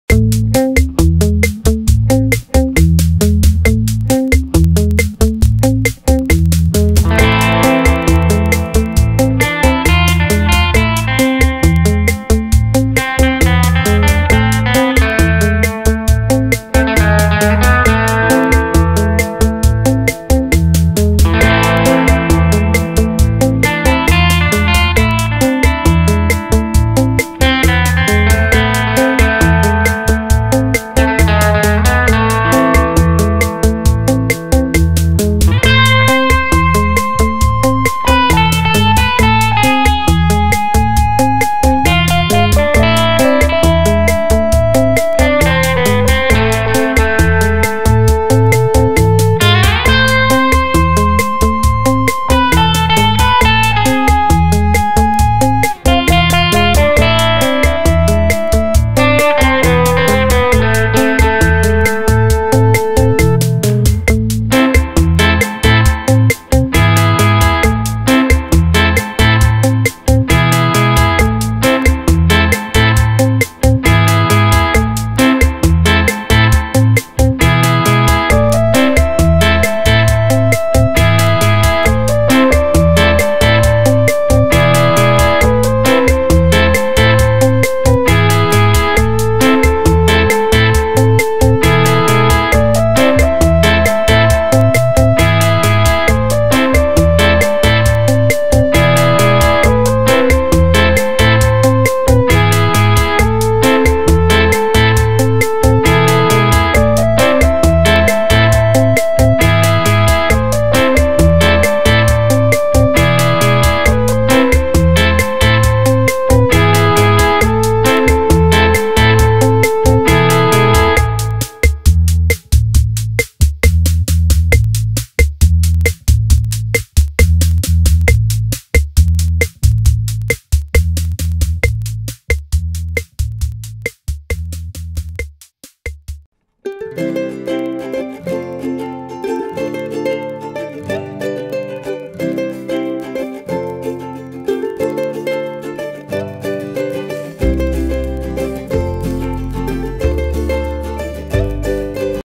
Easy DIY Wood Signs. How to Build and Paint Wood Signs.
Hi guys! In this video I am painting and writing on wood , building beautiful  decor signs. Enjoy it!